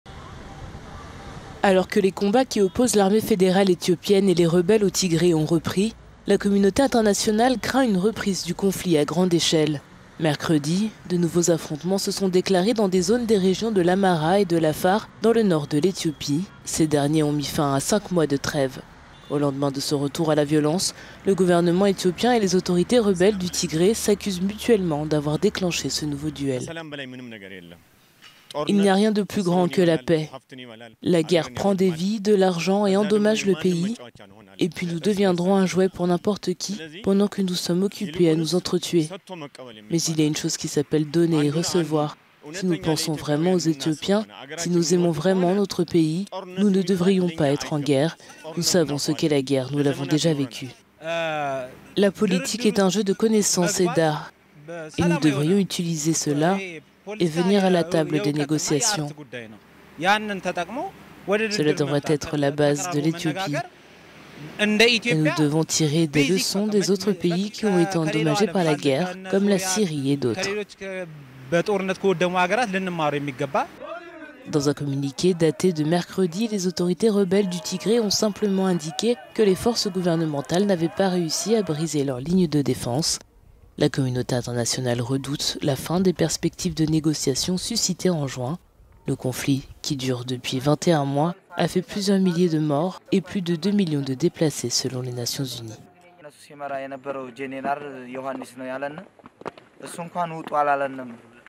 Éthiopie : la communauté internationale s'inquiète
Alors que les combats qui opposent l’armée fédérale éthiopienne et les rebelles au Tigré ont repris, la communauté internationale craint une reprise du conflit à grande échelle.